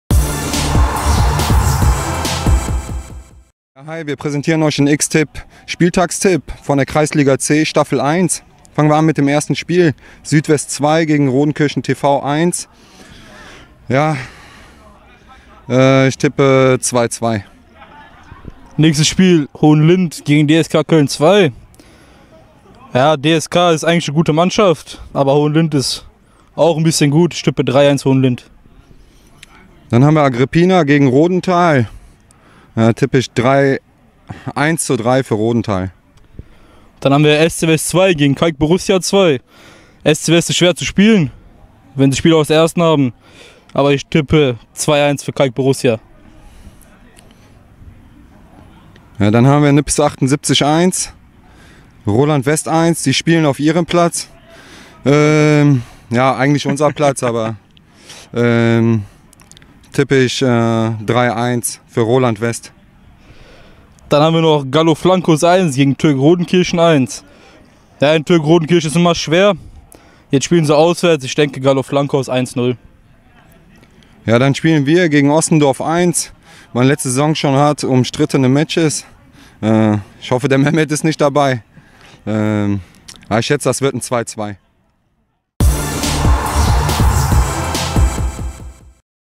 XTiP Spieltagstipp mit Muhamed Kucevic und Daniel Buschmann (Nippes GW II) - 2. Spieltag, KL C 1
XTiP Spieltagstipp mit Muhamed Kucevic und Daniel Buschmann (beide Nippes GW II) - 2. Spieltag, Kreisliga C, Staffel 1)

Muhamed Kucevic und Daniel Buschmann (beide Nippes GW II)  tippen den 2. Spieltag der Kreisliga C, Staffel 1. Stimmt Ihr zu oder liegen sie daneben?

RHEINKICK.TV und XTiP Sportwetten suchen den Tippkönig! Macht mit und bewerbt Euch. Dem Gewinner winkt am Ende der Saison eine attraktive Prämie.